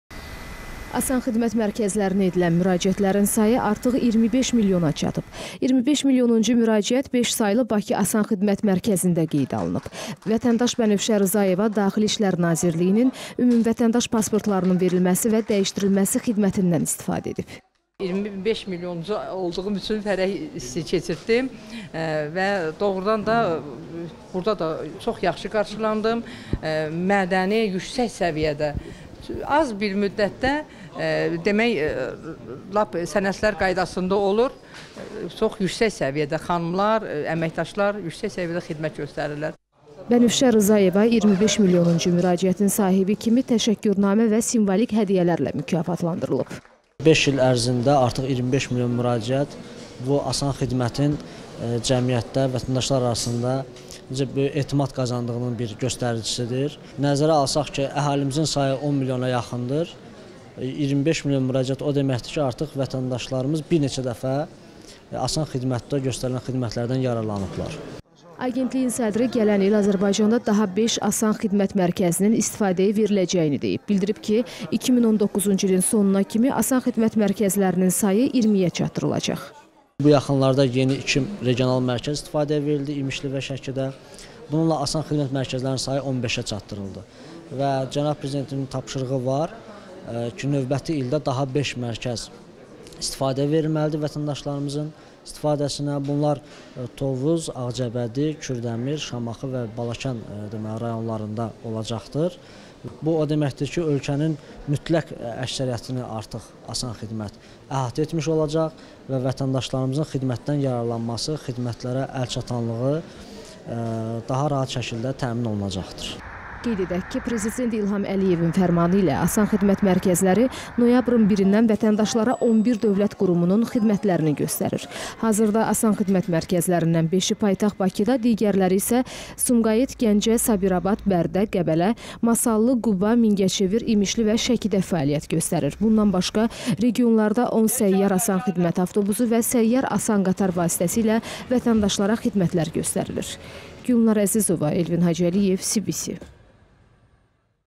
“ASAN XİDMƏT”Ə 25 MİLYONUNCU MÜRACİƏT QEYDƏ ALINIB
“ASAN xidmət” mərkəzlərinə edilən müraciətlərin sayı artıq 25 milyona çatıb. 25 milyonuncu müraciət  5 saylı Bakı “ASAN xidmət” mərkəzində qeydə alınıb. Vətəndaş  Bənövşə Rzayeva Daxili İşlər Nazirliyinin  ümumvətəndaş pasportlarının verilməsi və dəyişdirilməsi xidmətindən istifadə edib.
Bənövşə Rzayeva 25 milyonuncu müraciətin sahibi kimi təşəkkürnamə və simvolik hədiyyələrlə mükafatlandırılıb.
Agentliyin sədri gələn il Azərbaycanda daha 5 ASAN xidmət mərkəzinin istifadəyə veriləcəyini deyib. Bildirib ki, 2019-cu ilin sonuna kimi ASAN xidmət mərkəzlərinin sayı 20-ə çatdırılacaq.
Qeyd edək ki, prezident İlham Əliyevin fərmanı ilə ASAN xidmət mərkəzləri noyabrın 1-dən vətəndaşlara 11 dövlət qurumunun xidmətlərini göstərir. Hazırda “ASAN xidmət” mərkəzlərindən 5-i paytaxt Bakıda, digərləri isə Sumqayıt, Gəncə, Sabirabad, Bərdə, Qəbələ, Masallı, Quba, Mingəçevir, İmişli və Şəkidə fəaliyyət göstərir. Bundan başqa, regionlarda 10 Səyyar “ASAN xidmət” avtobusu və Səyyar "ASAN qatar" vasitəsilə vətəndaşlara xidmətlər göstərilir.